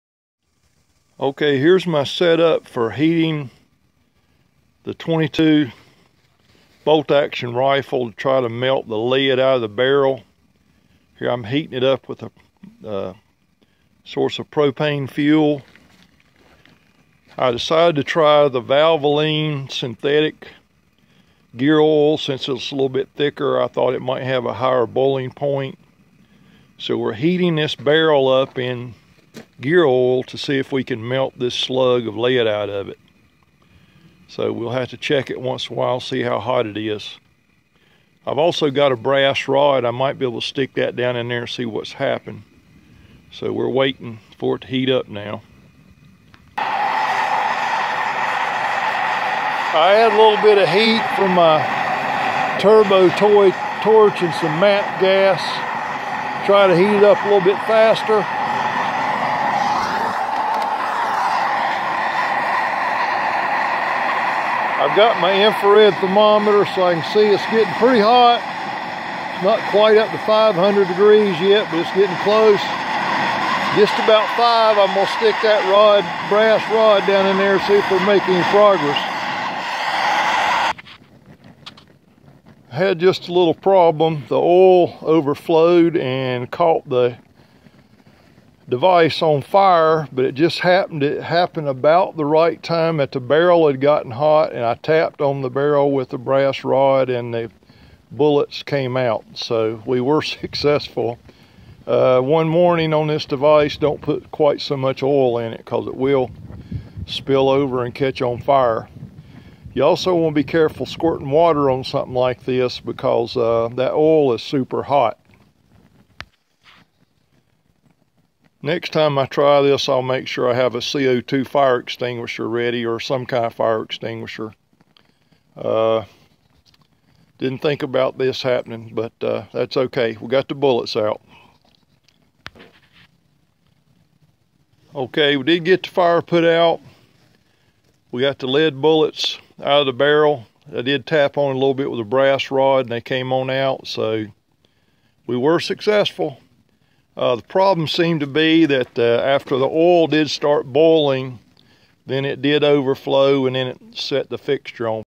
Removing a stuck bullet from a rifle barrel: Part 2
Melting  bullets stuck in a rifle barrel, without damaging the bluing.

The boiling point of gear oil is just a little higher than the melting point of lead.  After the gear oil caught fire and doing a little more research, I found out that the boiling point of paraffin is about 698 F, which is quite a bit higher than the melting point of lead.   If I ever have to do this again, I will use paraffin as the heating medium, instead of gear oil.  Any paraffin residue left on the barrel after can be melted off with a heat gun, since the melting point of paraffin is only about 99 F.   Make sure you have a CO2 fire extinguisher on hand if you are going to try this and do it outside, and away from your shop.   A CO2 fire extinguisher will put out the fire without contaminating your equipment with chemicals.